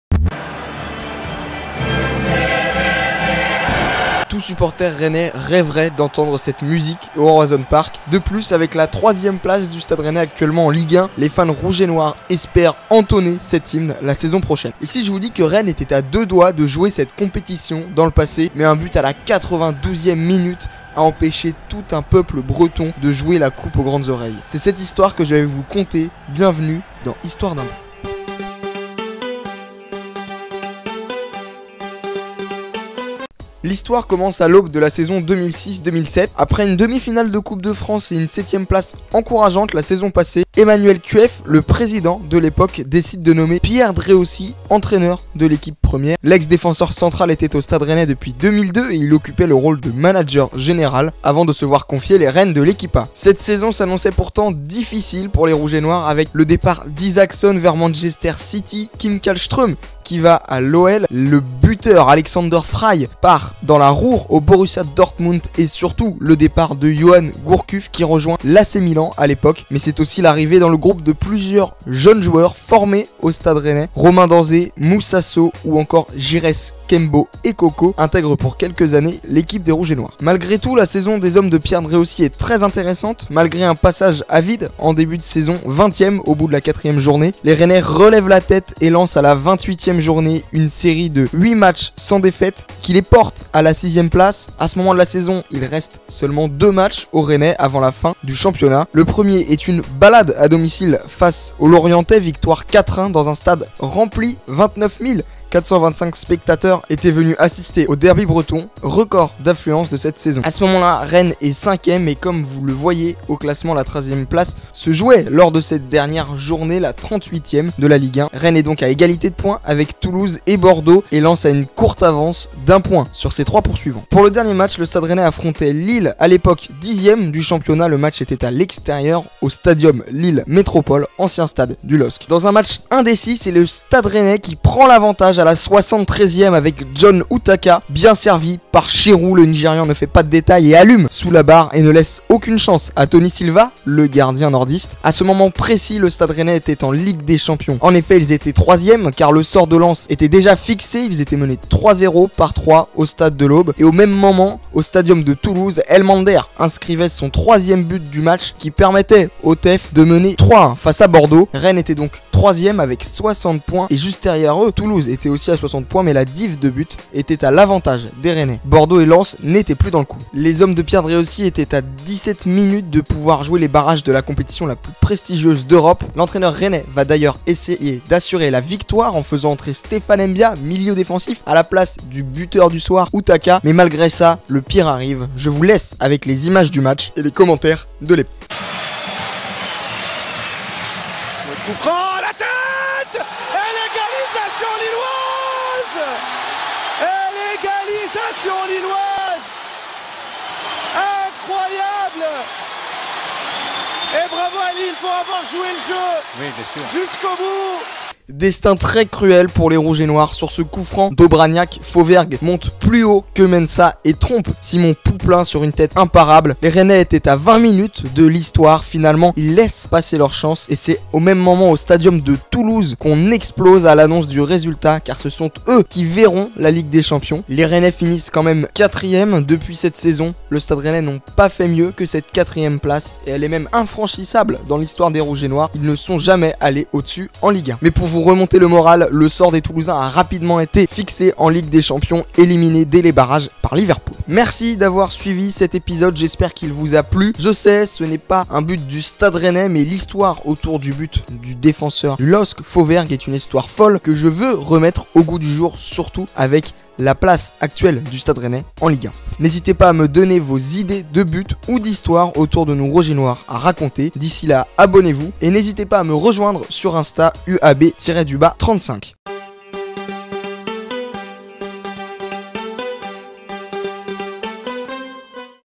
Quand le Stade Rennais était en ligue des champions (HBD #2)
Aujourd'hui je vous conte l'histoire de la saison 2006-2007 du Stade Rennais marquée par une une grande désillusion. Bon visionnage à tous.